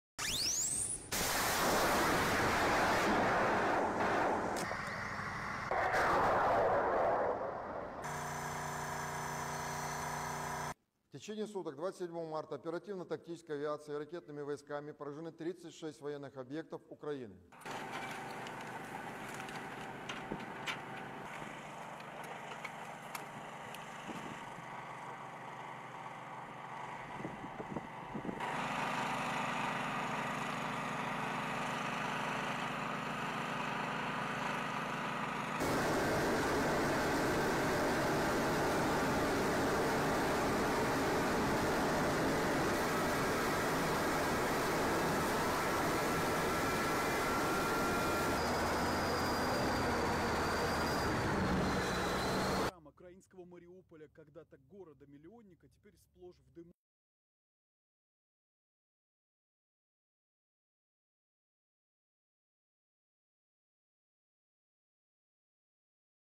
Live update rusia ukraina conflict: russian missile destroy ammunition depot in northern ukraine
𝙍𝙪𝙨𝙨𝙞𝙖𝙣 𝙢𝙞𝙨𝙨𝙞𝙡𝙚 '𝙙𝙚𝙨𝙩𝙧𝙤𝙮𝙨 𝙖𝙢𝙢𝙪𝙣𝙞𝙩𝙞𝙤𝙣 𝙙𝙚𝙥𝙤𝙩' 𝙞𝙣 𝙣𝙤𝙧𝙩𝙝𝙚𝙧𝙣 𝙐𝙠𝙧𝙖𝙞𝙣𝙚